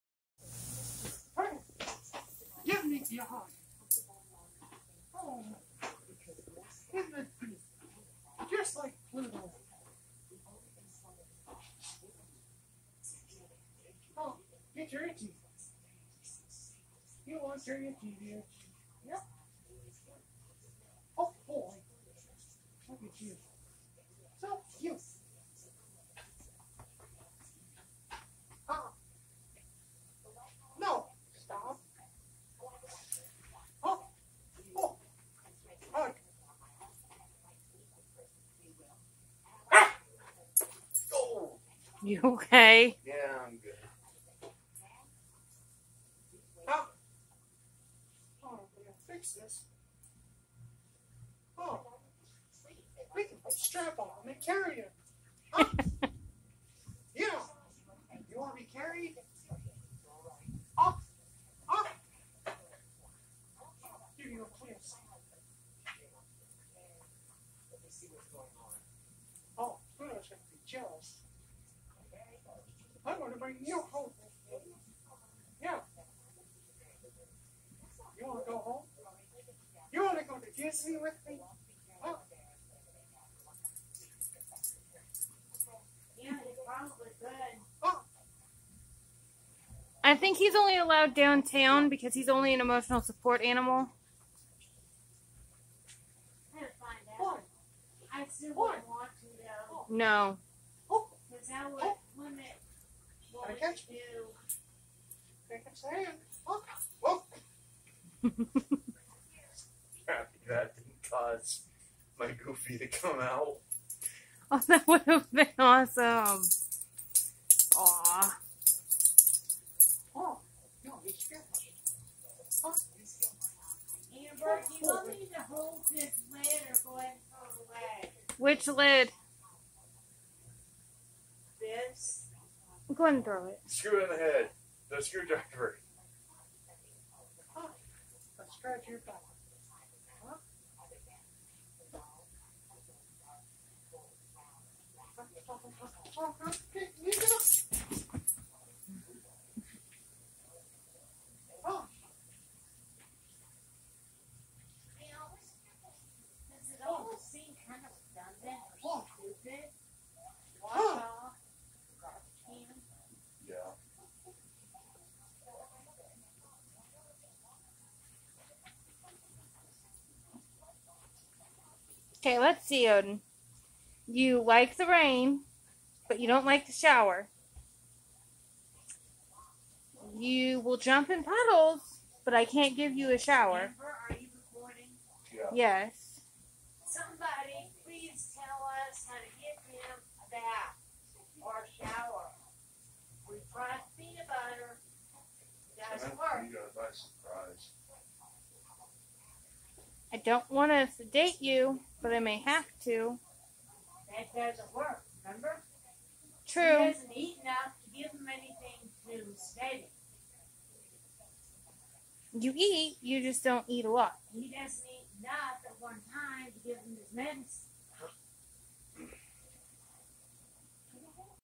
My brother doing his Mickey voice while playing with Odin
My brother does a great mickey mouse and doing responds when they play its great